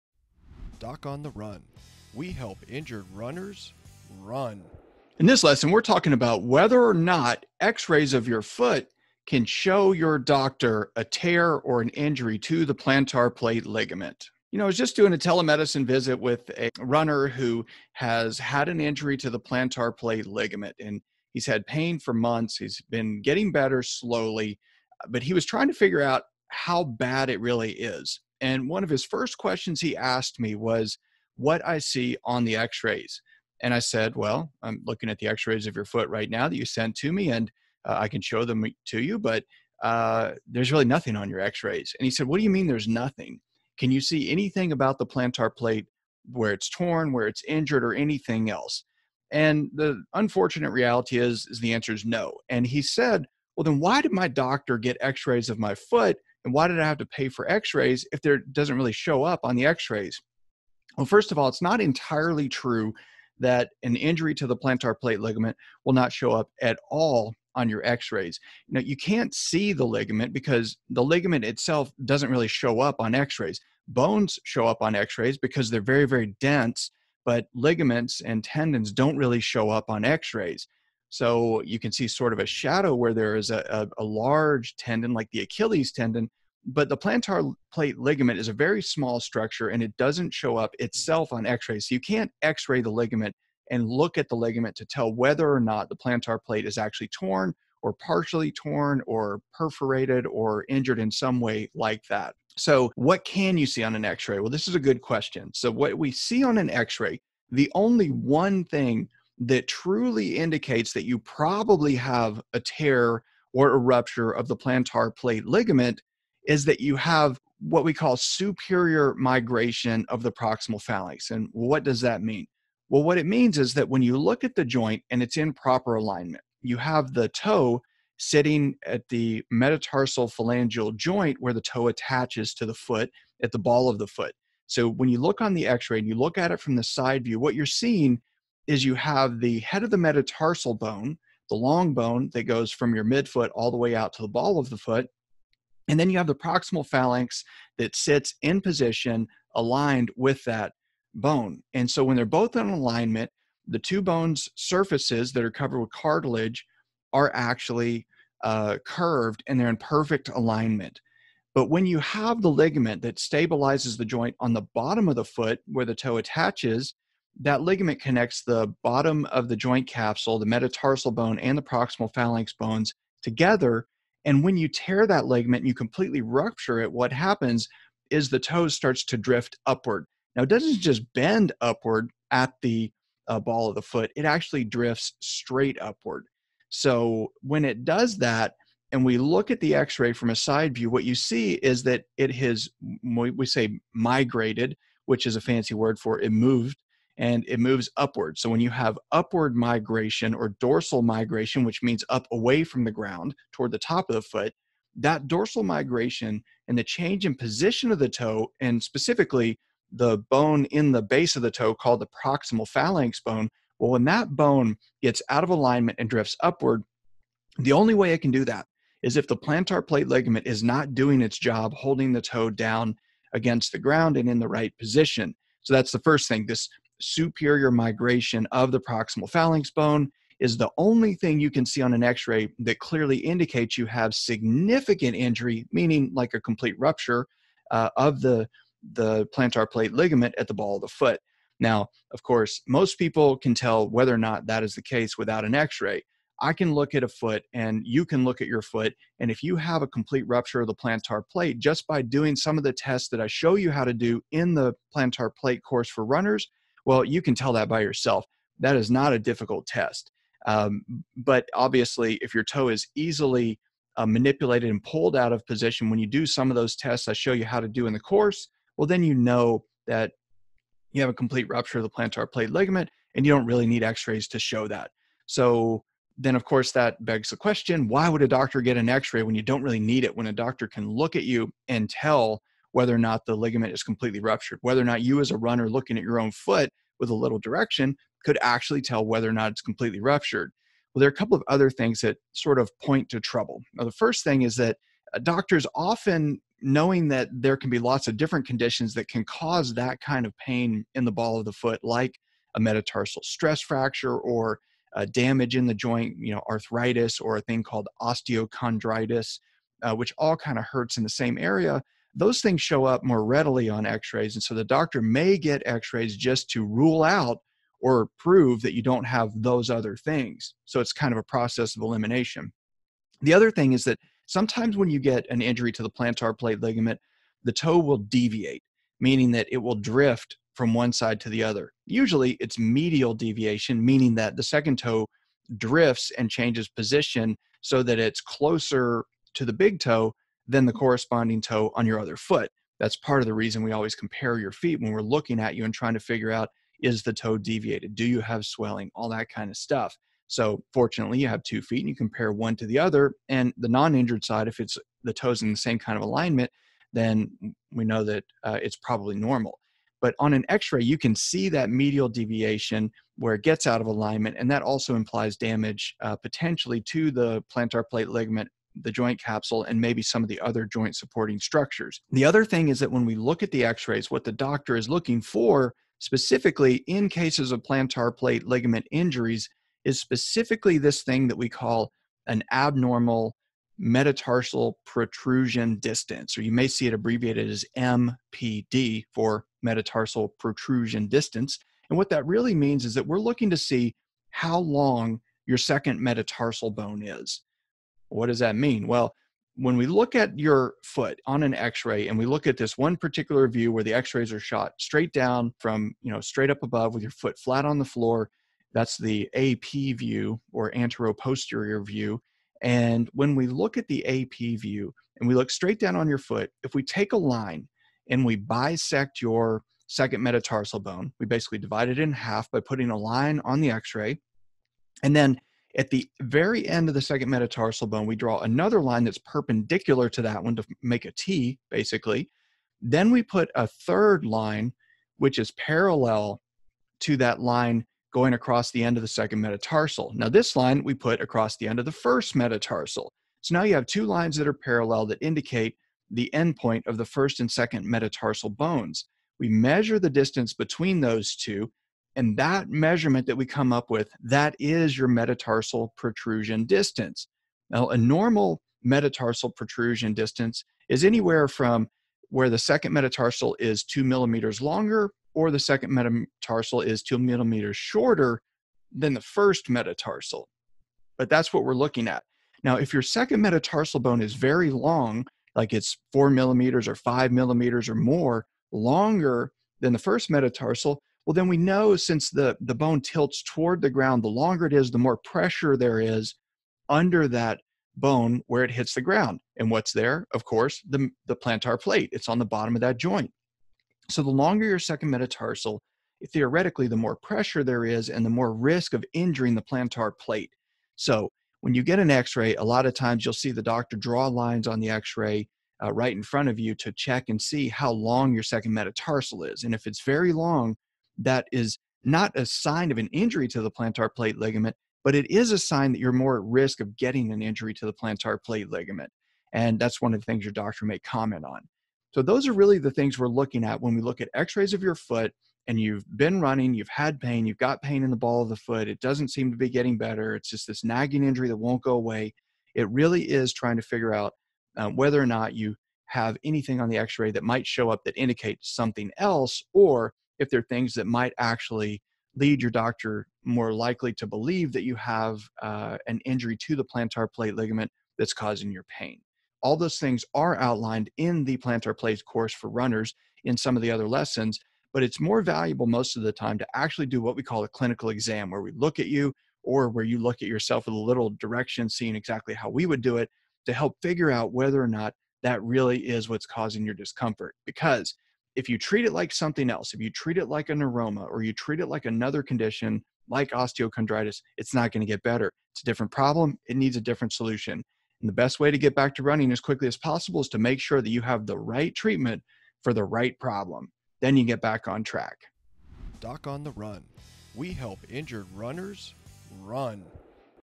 Can x-rays show a plantar plate injury?
This video comes from one of the lessons in the Plantar Plate Sprain Self-Diagnosis and Self-Treatment Course for Runners.

Plantar plate tears can be one of the most difficult running injuries to heal quickly. X-rays show bone well, but don't show ligaments very well. If you suspect you have a plantar plate sprain and go to the doctor, you may wonder whether or not plantar plate injury show up on x-ray at all. This video explains what is visible on an x-ray of your foot that may tell you if you are at risk of a plantar plate tear, or what changes on x-ray are visible when you have a completely torn plantar plate ligament. Once you understand that, you can make a decision about whether or not you need x-rays of your foot for a suspected plantar plate sprain in the ball of the foot.